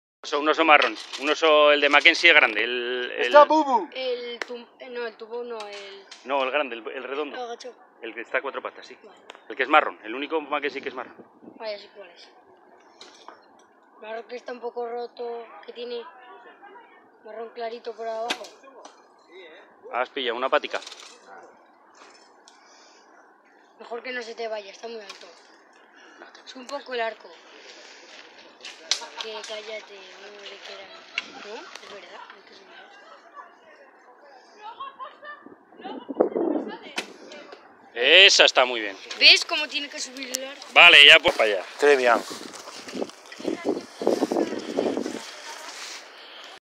Tirada de la Liga vasca 3D, Ordizia, 16/02/2020
Competencia de tiro con arco, instintivo, desnudo, Longbow y compuesto. En Gainza cerca de Ordizia en el campo de tiro 3D del club Ordizia, Guipuzcoa.